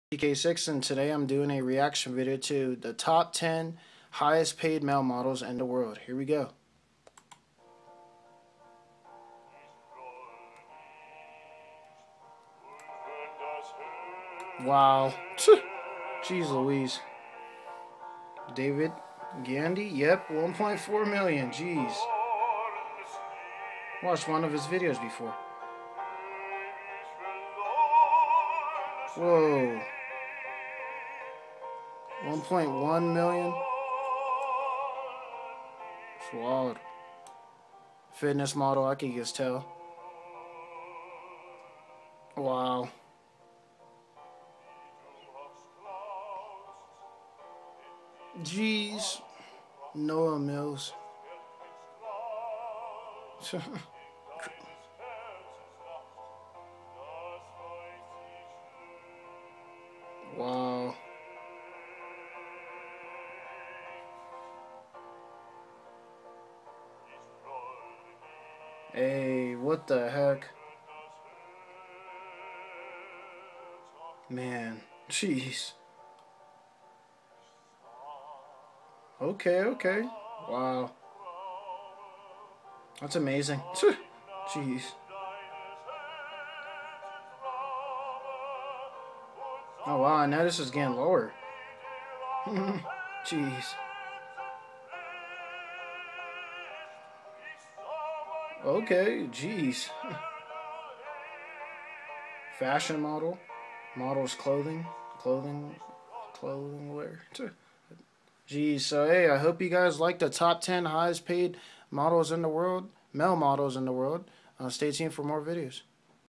A reaction video to The Top 10 Highest Paid Male Models in The World
Hope you guys like this video of the top male models in the world I was shocked of how much money some of these men get that's pretty wild davidgandy Tysonballou Sean O'Pry simonnessman ArthurKulkov Noahmills Ryan Burns Ollie Edwards Jon Kortajarena Tobias Sorensen